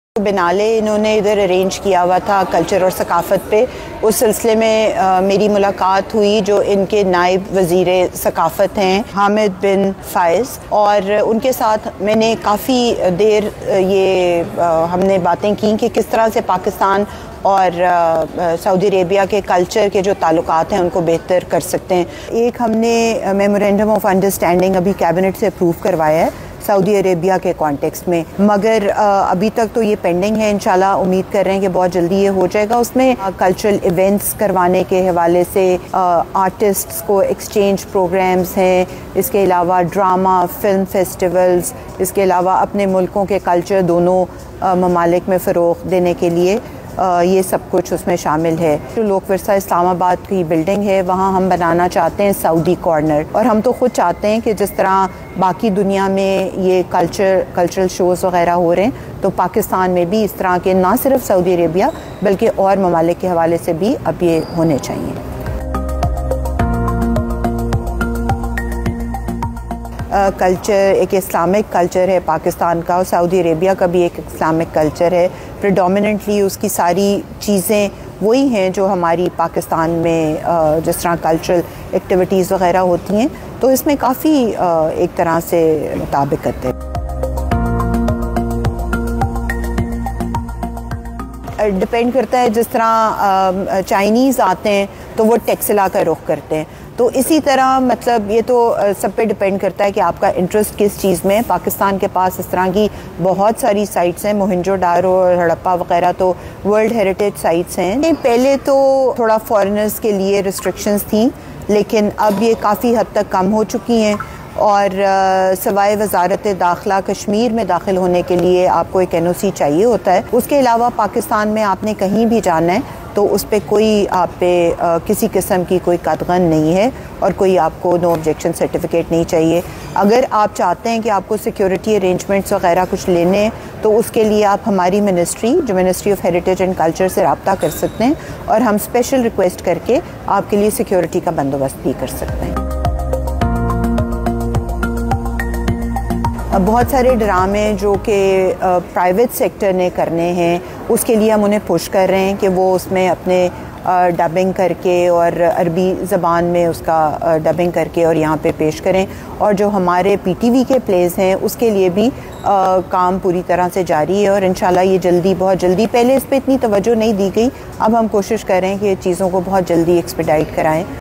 پاکستان کی سیکریٹری برائے قومی ورثہ و ثقافت فارینہ مظہر سعودی عرب کے دورہ پر
مزید دیکھیں ایاز چوہدری کا انٹرویو